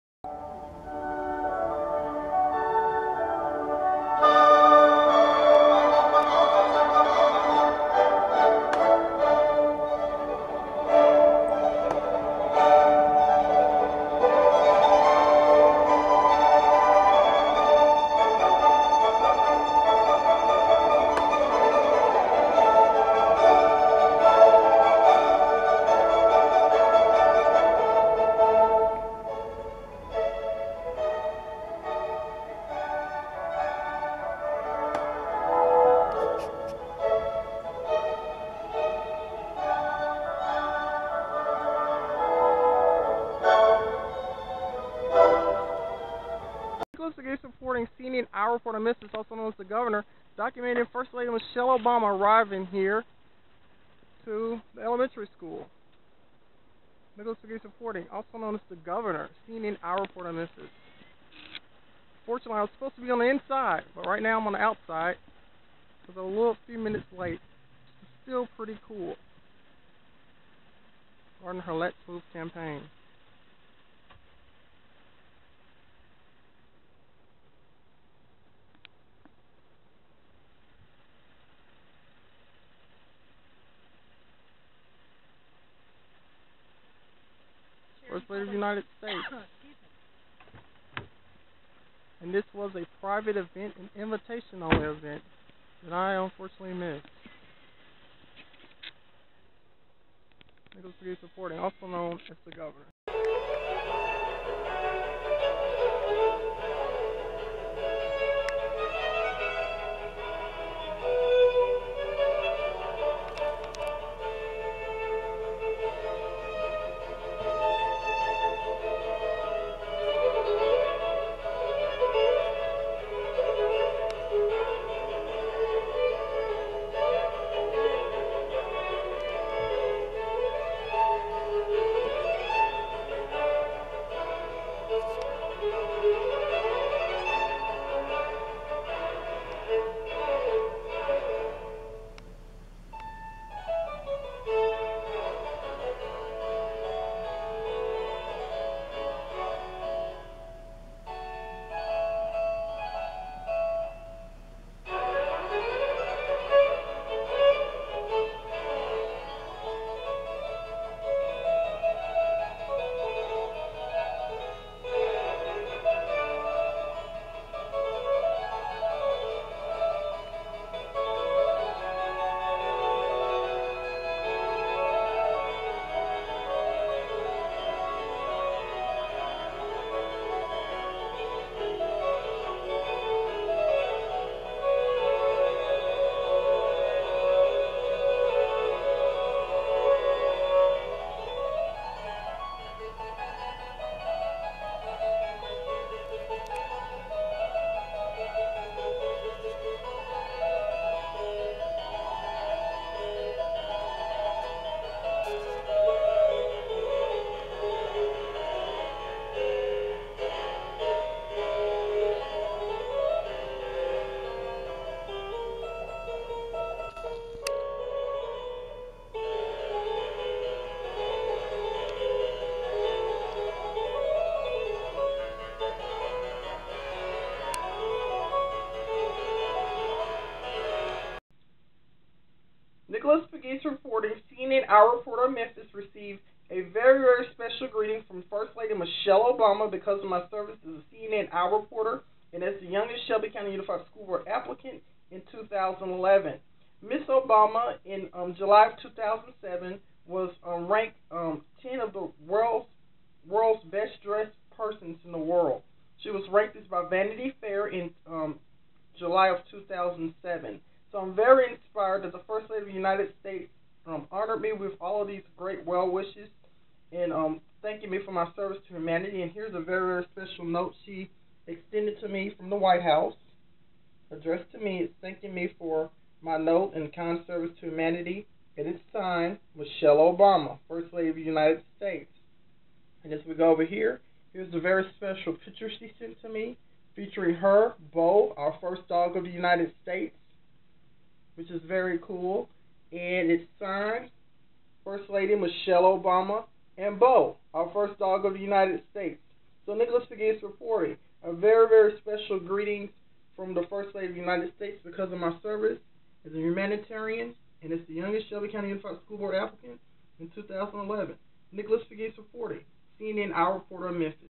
The White House - Mrs. Obama A Elected Fan of Nicholas Pegues The Governor for Cingress 2014
Office of the Press Secretary

For Immediate Release                                                                                                                                July 18, 2014

REMARKS BY THE PRESIDENT

AND THE FIRST LADY

AT KIDS STATE DINNER

East Room

12:17 P.M. EDT

     MRS. OBAMA:  Okay, Braeden.  (Laughter.)  All right, it's going to be hard.  All right, mister, you're not supposed to make the First Lady cry.  (Laughter.)
Thanks so much.  You guys, let's give Braeden a big round of applause.  (Applause.)  Well done, young man.  Well done.  And thank you so much, Christy.  Thank you.
     Okay, this is cool, right?  You guys having a good time?
  AUDIENCE:  Yes.
MRS. OBAMA:  Yes?  Has it been exciting from beginning to end?AUDIENCE:  Yes.
 MRS. OBAMA:  What's been the best part so far?
 AUDIENCE:  You.
MRS. OBAMA:  This?  All right, I didn't cue them on that one.  (Laughter.)  That was a natural response.  Well, I am just delighted to have you guys.  You guys look amazing.  You have dressed so appropriately for the event.  Doesn't the room look beautiful?  They do such a great job.  And all this stuff was picked out especially for you guys.  It is really an honor to be here.
     And I want to start by thanking Tanya and all the folks from Epicurious, the entire team, for, year after year, making this event possible.  It is truly the highlight of our lives, and I know that everyone involved really, really gets inspired from this event.
And I'm also thrilled about the new Kids and Chefs Cook for Success program that you guys are launching.  And with this effort, you guys are going to take this whole thing to another level.  So I love the fact that, Epicurious, you're finding ways to step this up every single year.  So thank you, guys, and a big round of applause.  (Applause.)